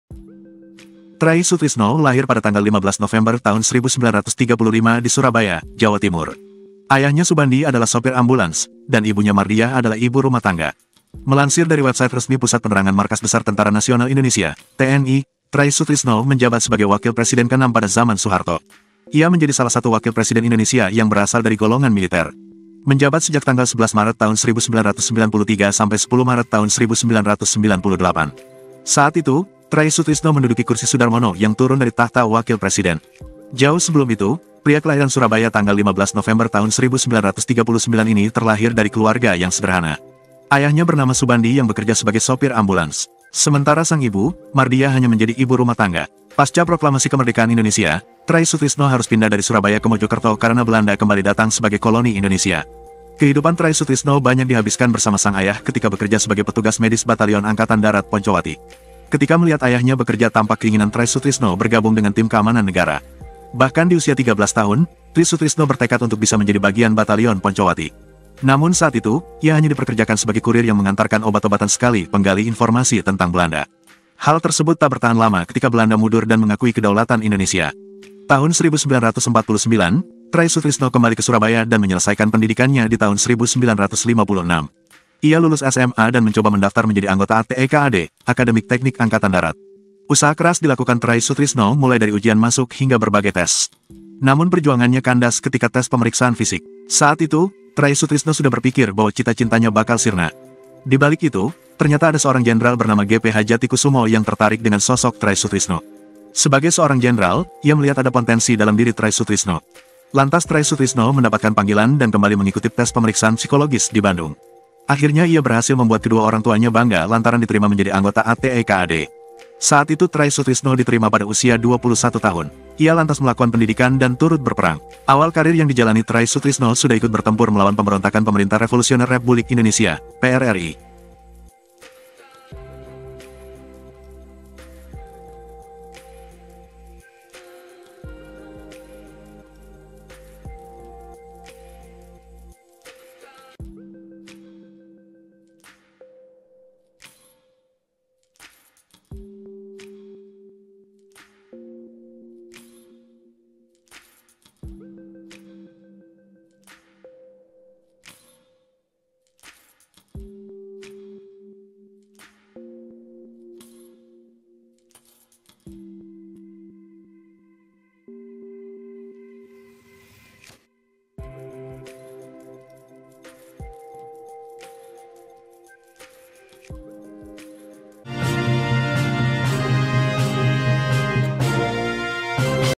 Profil Try Sutrisno, Anak Supir Ambulans Jadi Wakil Presiden ke 6 Republik Indonesia
Try Sutrisno lahir pada 15 November 1935 di Surabaya, Jawa Timur.

Ayahnya Subandi adalah sopir ambulans, dan ibunya Mardiyah adalah ibu rumah tangga.

Melansir dari websit resmi Pusat Penerangan Markas Besar Tentara Nasional Indonesia (TNI), Try Sutrisno menjabat sebagai wakil presiden ke-6 pada zaman Soeharto.

Ia menjadi salah satu Wakil Presiden Indonesia yang berasal dari golongan Militer.

Menjabat sejak 11 Maret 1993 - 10 Maret 1998. Saat itu, Try Sutrisno menduduki kursi Sudharmono yang turun dari tahta wakil presiden.

Jauh sebelum itu, pria kelahiran Surabaya 15 November 1939 ini terlahir dari keluarga yang sederhana.

Ayahnya bernama Subandi yang bekerja sebagai sopir ambulans. Sementara sang ibu, Mardiya hanya menjadi ibu rumah tangga.

Pasca Proklamasi Kemerdekaan Indonesia, Try Sutrisno harus pindah dari Surabaya ke Mojokerto karena Belanda kembali datang sebagai koloni Indonesia.
Editor: Ikr